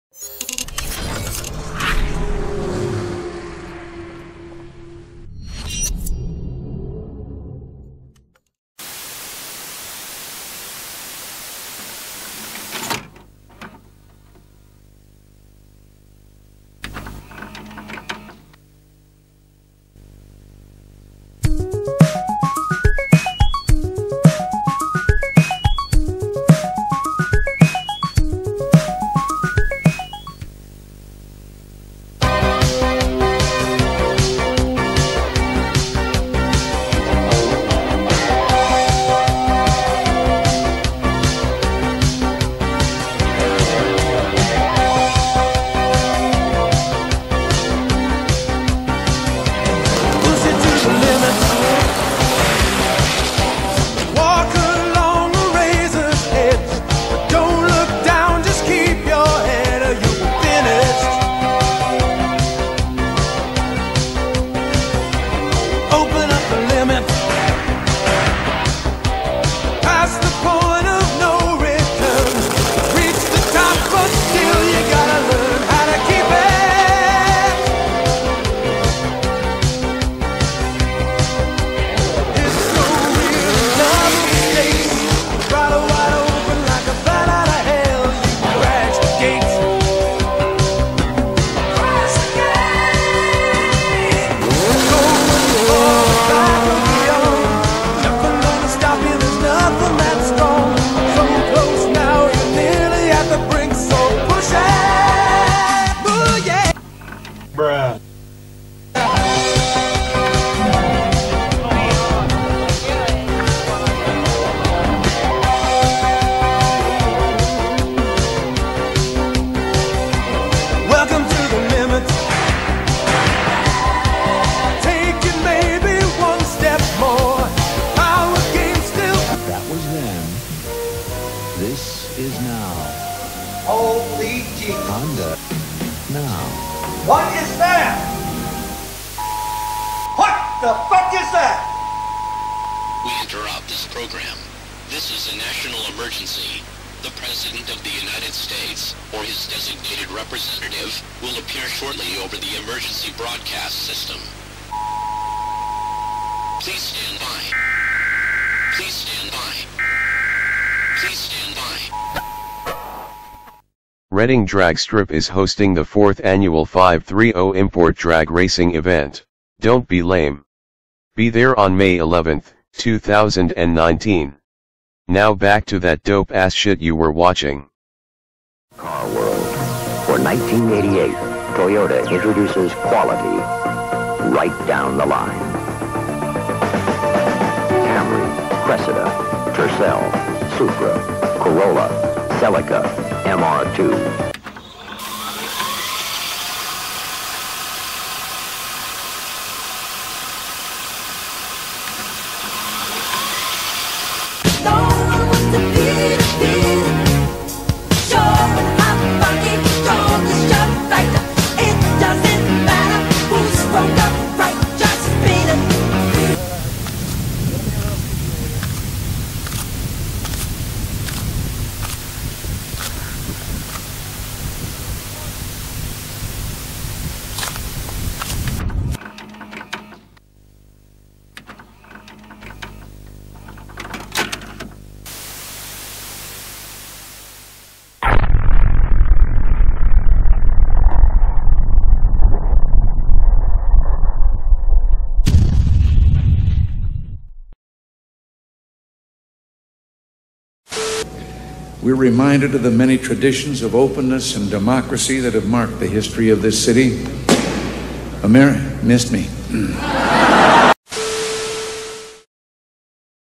Five30 Import Drags IV Promo
five30 import drags round 4 announced. may 11, 2019 in redding ca. 1988 called... they want their cheesy effects back hahaha